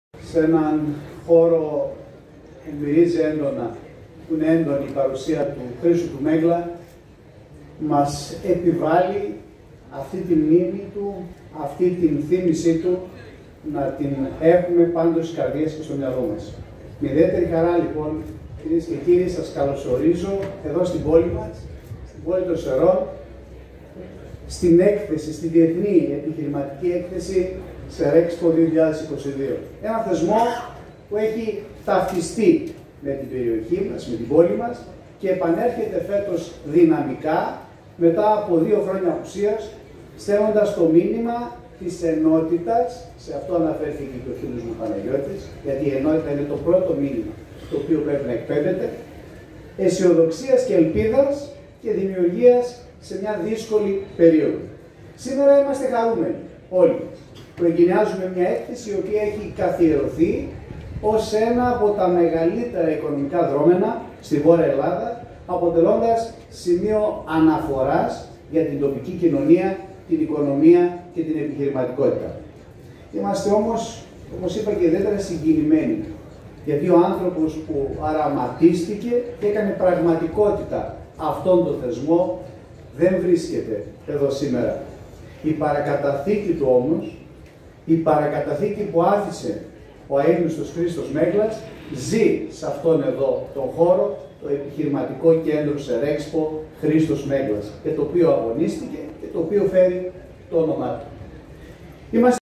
Ομιλία του δημάρχου Σερρών Αλέξανδρου Χρυσάφη στην Serexpo
Πρώτη φορά συνδιοργανωτής της, SEREXPO είναι ο Δήμος Σερρών, με τον Δήμαρχο Αλέξανδρο Χρυσάφη να τονίζει τη σπουδαιότητα του γεγονότος για την ενίσχυση των επιχειρήσεων.